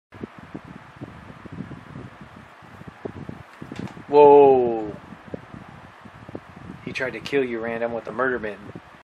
Ash punch through the window
Sorry Random Ash didn't like what he saw out his window.